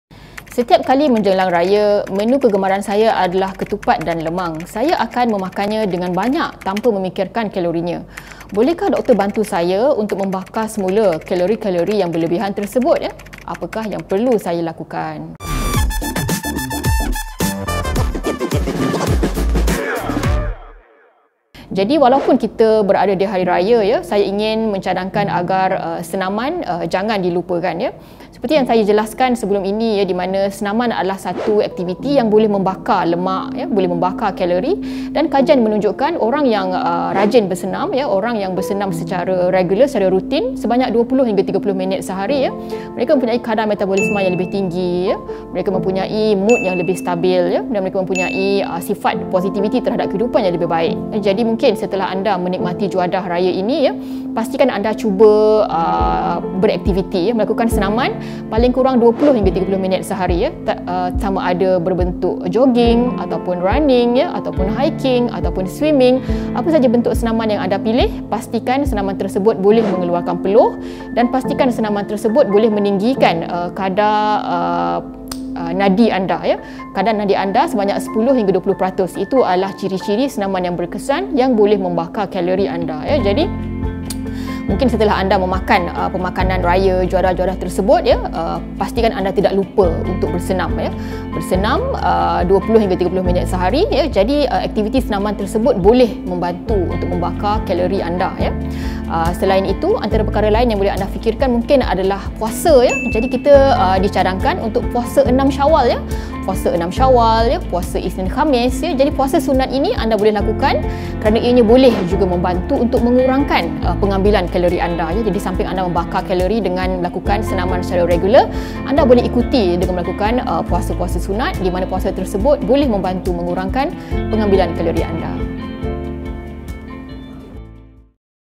Lebaran Dan Kalori - Soal Jawab Netizen Medik Edisi Ramadan
Ramai yang kata, sempatkah nak kira kalori kalau menjamah apa-apa makanan?

Kata mereka, kalau terlalu berkira-kira, nikmat makan akan hilang.

Namun, ada petua sebenarnya untuk kekalkan berat badan walau pada musim perayaan.

Jom dengarkan tip yang dikongsikan Pakar Perunding Perubatan, Paru-paru dan Permasalahan Tidur, Pusat Perubatan Sunway Velocity, Dr Nurul Yaqeen Mohd Esa dalam Soal Jawab Netizen Medik Ramadan ada jawapannya.